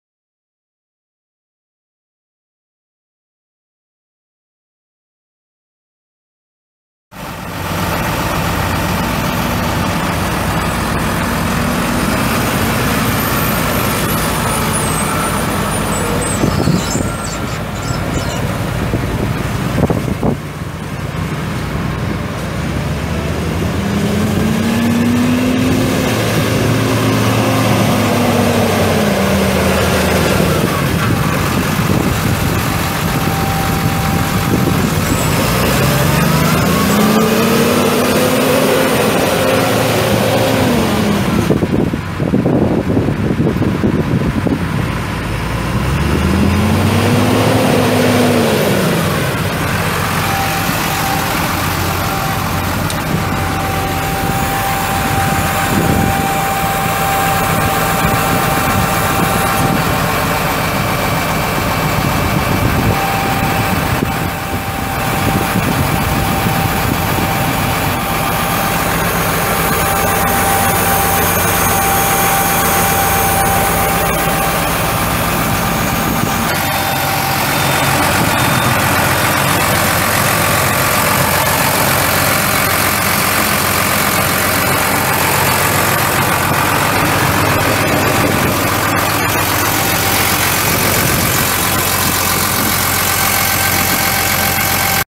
2015 Case IH Farmall 140A MFWD tractor for sale | no-reserve Internet auction March 14, 2018
View a detailed listing of this item and place bids

2015 Case IH Farmall 140A MFWD tractor
5,665 hours on meter
FPT 6.7L six cylinder diesel engine
Power shuttle shift transmission
8F-8R gears
Enclosed cab
AC and heat
Work lights
540 PTO
(2) rear hydraulic remote sets
18.4-38 rear tires
Serial CF02497M